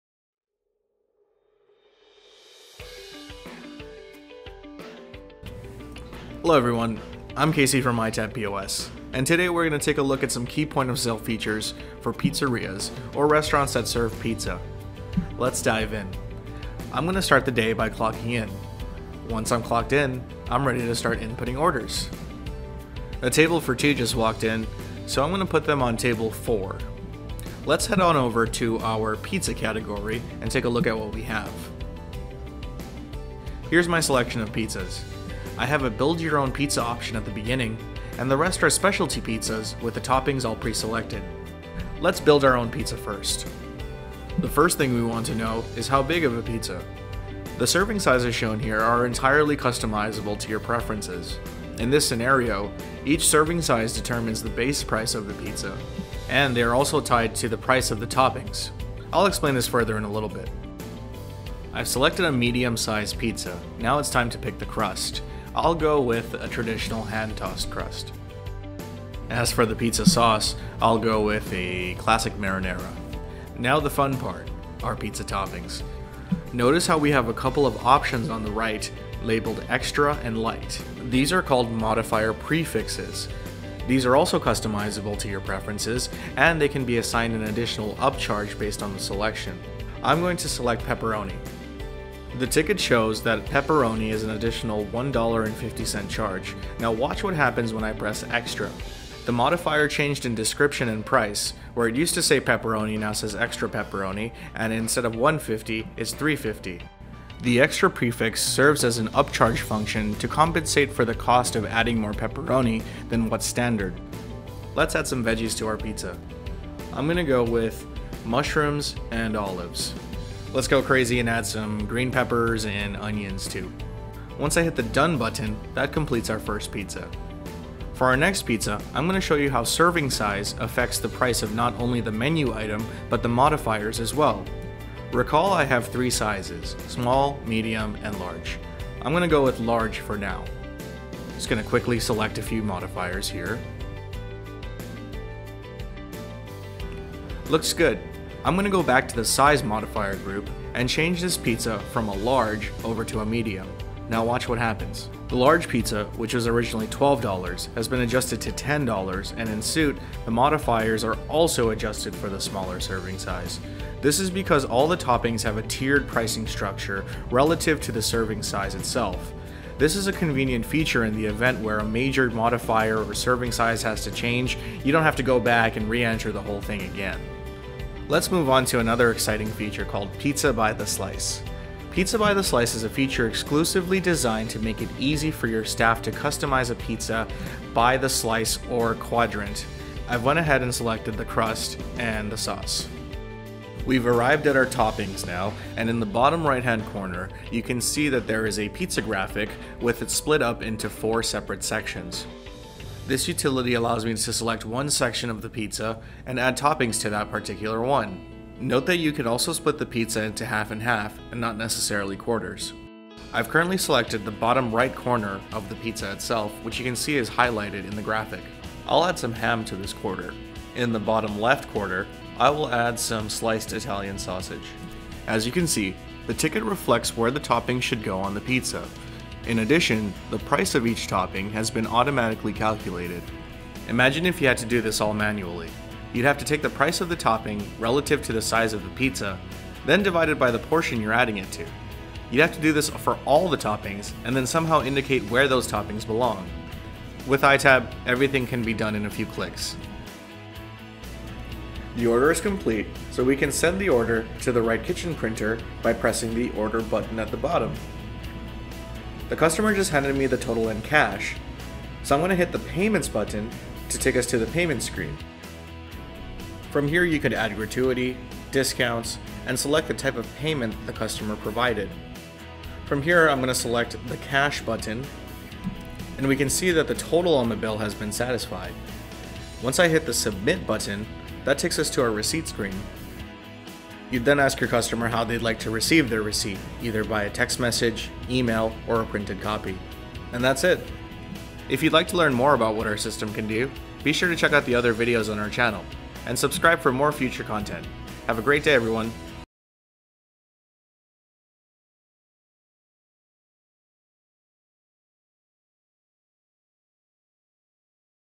Pizzeria POS - iTab POS
to learn more about our iPad POS system built for pizzerias or restaurants that serve up pizza. Our Pizza POS system features enable the waitstaff, kitchen workers, managers, and owners to work and communicate efficiently. You can learn more by visiting our website or calling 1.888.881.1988 today!